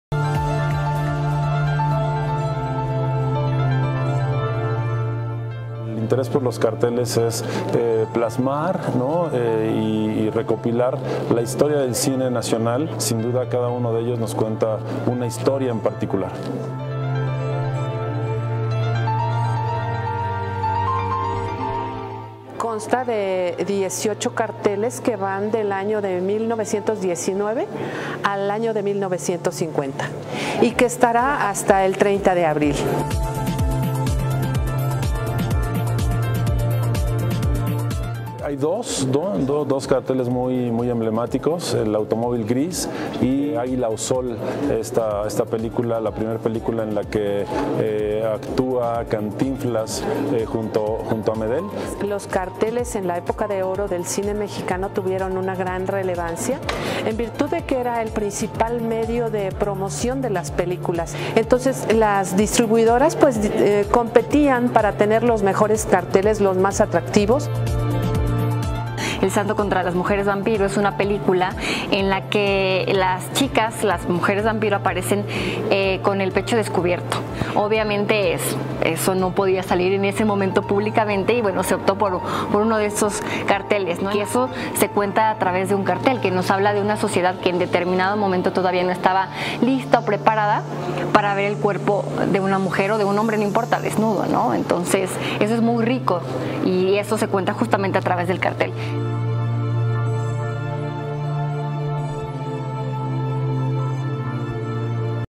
Gastón Pedraza Exposición de carteles mexicanos
“El Santo”, “Cantinflas” y Pedro Infante, están en la Cineteca Mexiquense como parte de la exposición “Carteles del Cine Mexicano” que muestra piezas históricas de grandes películas de épocas del séptimo arte.

La más antigua es “El automóvil gris” de 1919, y cuenta con los carteles originales de grandes películas como: “Ustedes los Ricos”, “El Santo vs las mujeres vampiros” y “Aguila o sol” .

La exposición la comparte Gastón Pedraza, director general de comunicación universitaria de la UAEMéx, quien ha recabado estas piezas únicas a lo largo de los años.

Lánzate a la Cineteca para conocer otra faceta de la industria fílmica del siglo pasado en Mexico, pues cada cartel es por sí mismo, una obra de arte y reflejan la vida y estilo de diferentes décadas de México.